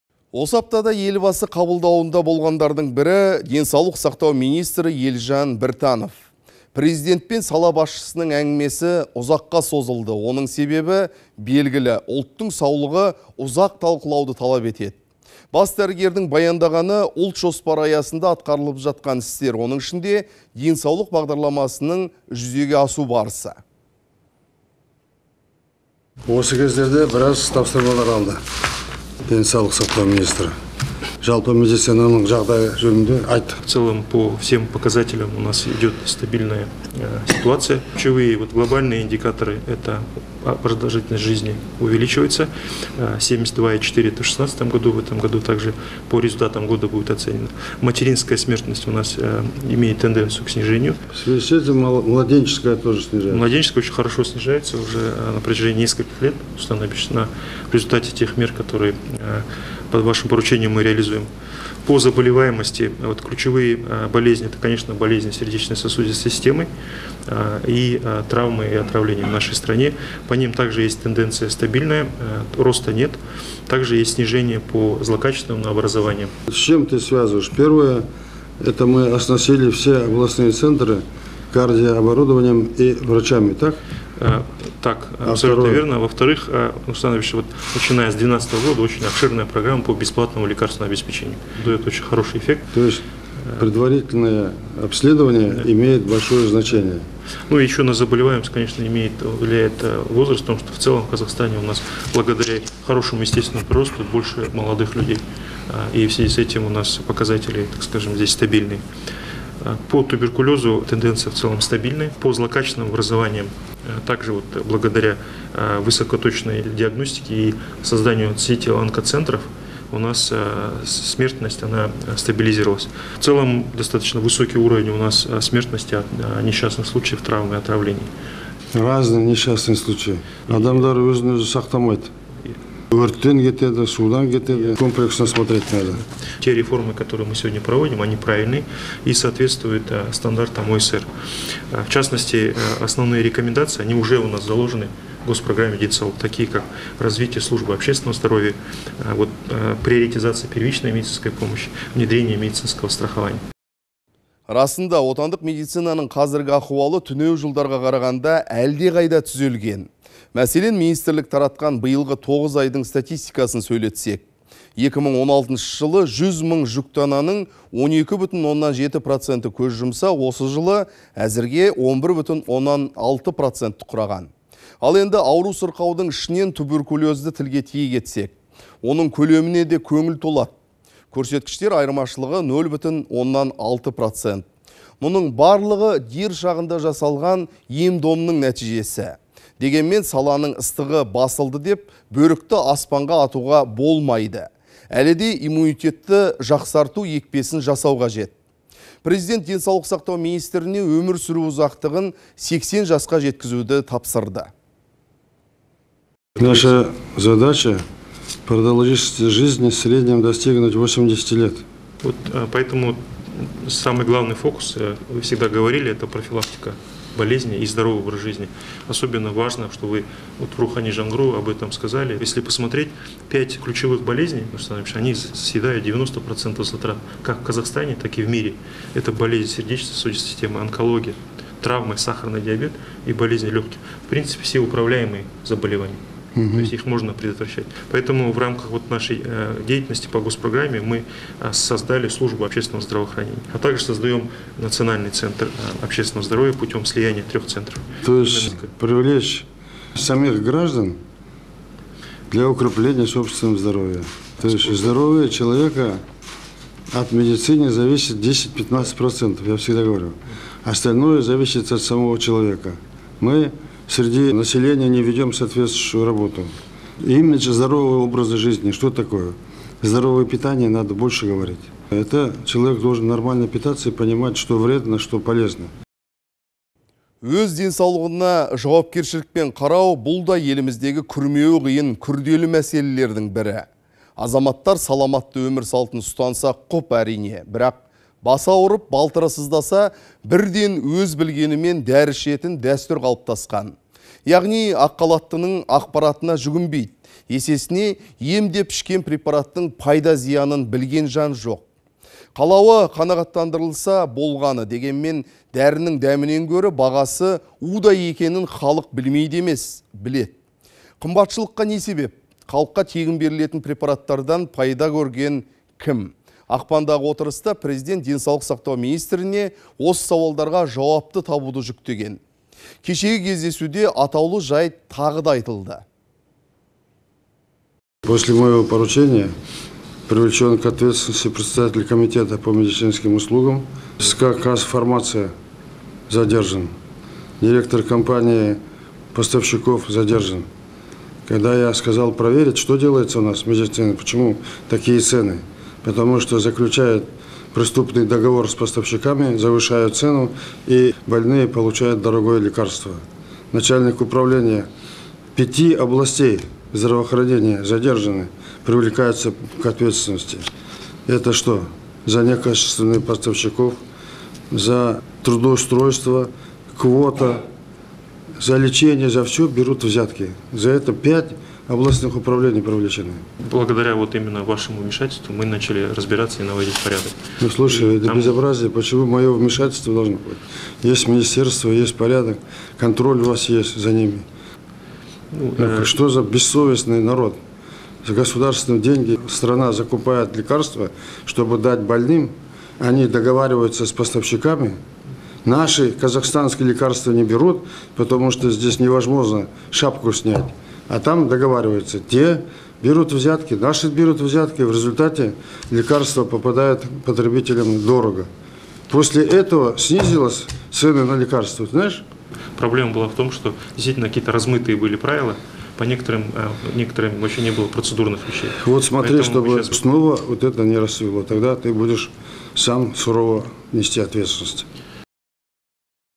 Денсаулық сақтау саласындағы мәселелер
Осы аптада Елбасы қабылдауында болғандардың бірі – Денсаулық сақтау министрі Елжан Біртанов. Президент пен сала басшысының әңгімесі ұзаққа созылды. Оның себебі – ұлттың саулығы «ұзақ талқылауды» талап етеді.

Бас дәрігердің баяндағаны «Ұлт жоспары» аясында атқарылып жатқан істер, оның ішінде «Денсаулық» бағдарламасының жүзеге асу барысы.

Елжан Біртанов ҚР Денсаулық сақтау министрі:

-По всем показателям ситуация стабильная. Ключевые, глобальные индикаторы, как продолжительность жизни увеличивается. 72,4 в 2016 году, в этом году также по результатам году будет оценена. Материнская смертность имеет тенденцию к снижению. Младенческая смертность также снижается на протяжении уже нескольких лет, в результате тех мер, под вашим поручением мы реализуем. По заболеваниям, ключевые болезни это сердечно-сосудистой системы, травмы и отравления в нашей стране. По ним так же есть тенденция снижения, роста нет. Далее есть снижения по злокачественным образованиям.